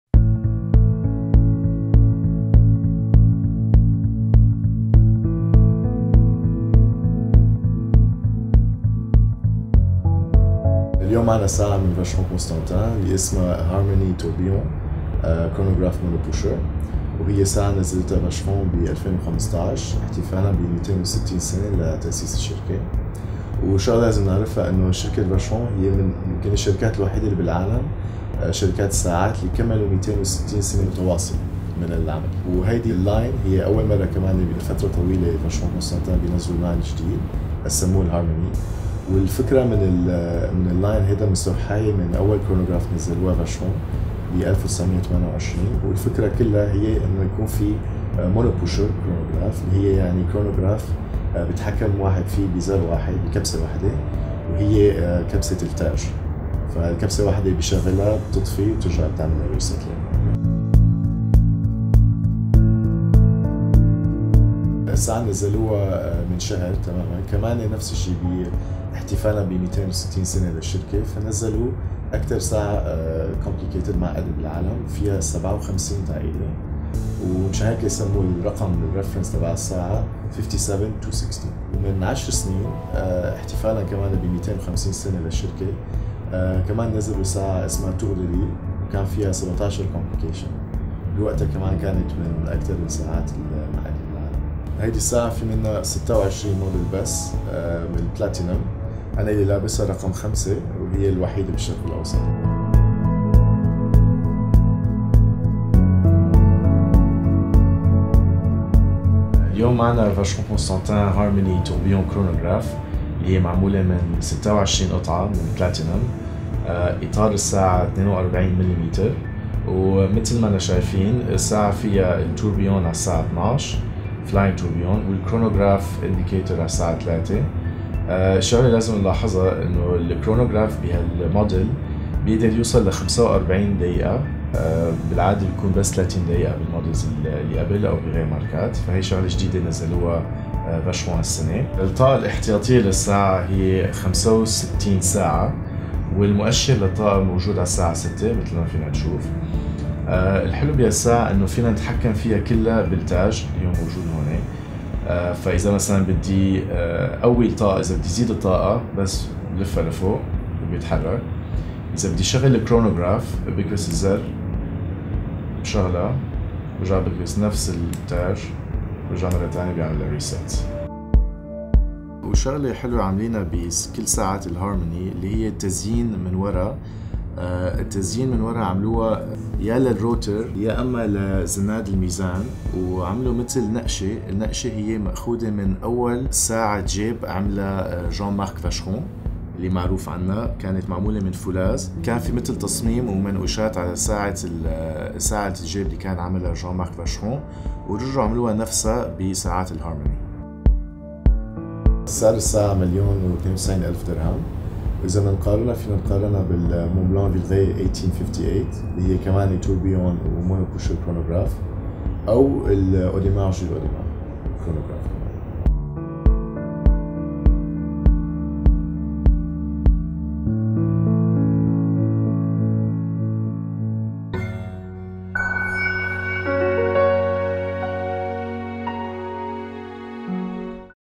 AWG reviews Vacheron Constantin Harmony Tourbillon Chronograph
تقديم و شرح مفصل لساعة مميزة جدا من شركه فاشرون كونستانتين